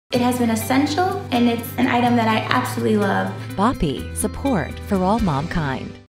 Boppy® Feeding & Infant Support Pillow
The original, award-winning Boppy® Feeding and Infant Support Pillow celebrates nearly three decades of supporting moms, dads and babies! The Boppy® Pillow not only lifts babies to a more ergonomic position for comfortable feedings, but also transitions to support them as they grow offering the perfect spot for tummy time, playing, sitting or just discovering little fingers and toes. Have an extra nursing pillow slipcover on hand for in-between wash cycles.